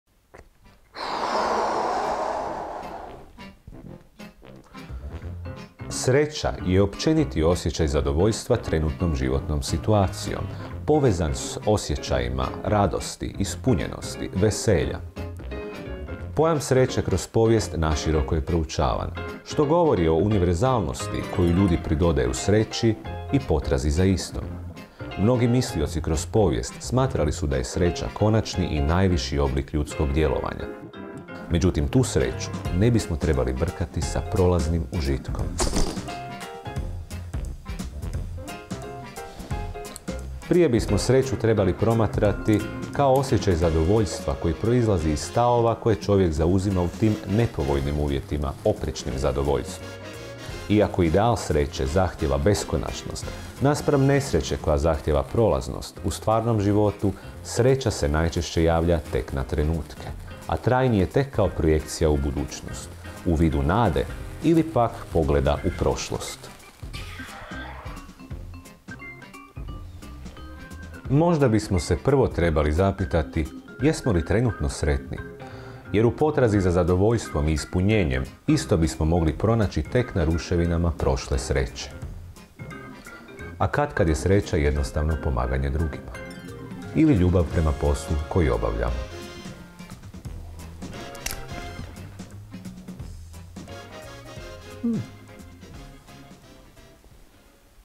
Trivijator: Sreća
Autor: Janko Debeljak
Snimatelj: Petar Kleščić

Pojam sreće kroz povijest je naširoko proučavan, što govori o univerzalnom značaju koji ljudi pridaju sreći i potrazi za istom. No, možda bismo se prvo trebali zapitati - jesmo li već sretni?